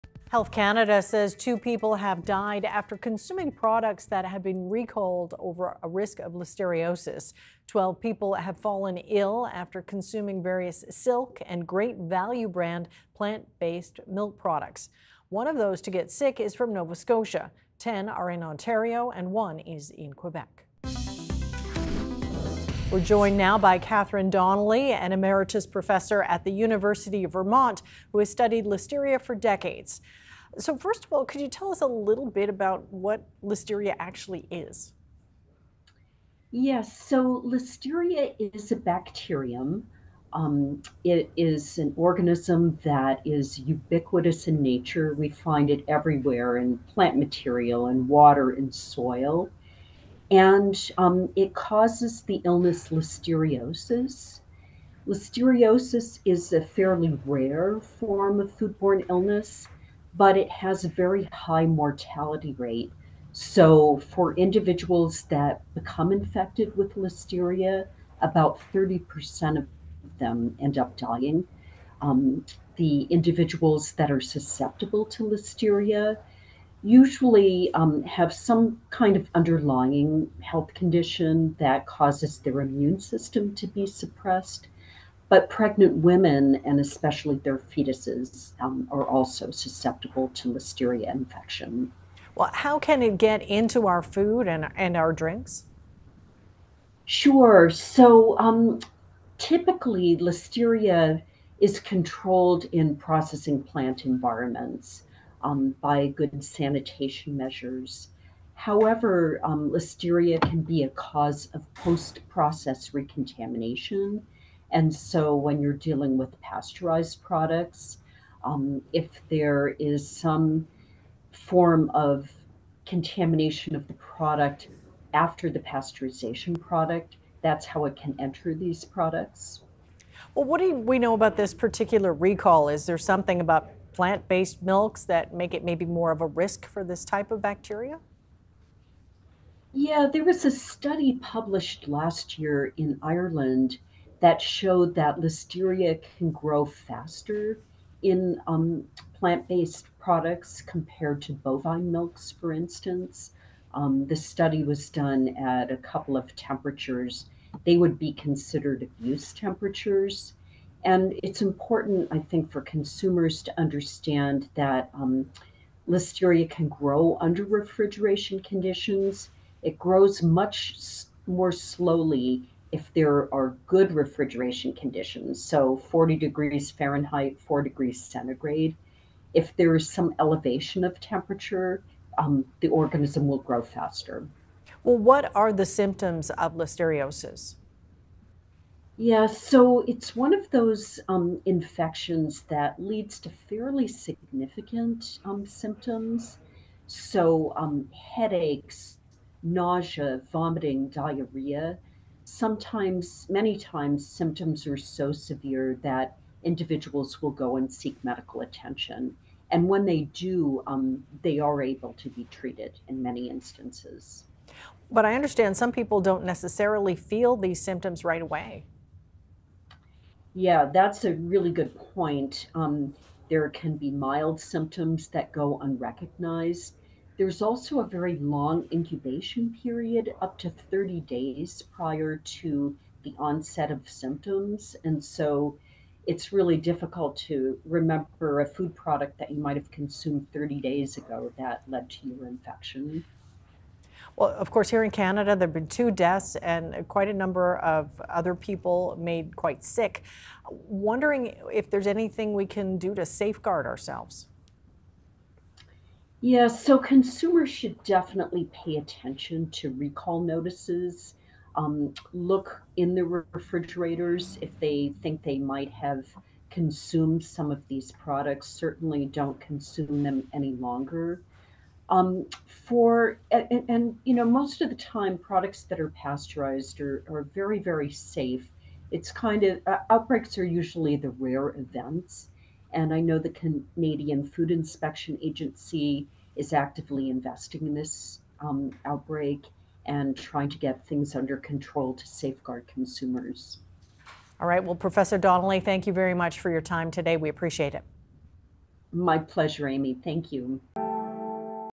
What a listeriosis expert says about the plant-based milk recall
Listeriosis linked to some Silk and Great Value plant-based milks resulted in two deaths in Ontario. Catherine Donnelly, a professor emeritus at the University of Vermont who has studied listeria for decades, says consumers should pay attention to recall notices and look in their refrigerators if they think they may have consumed some of these products.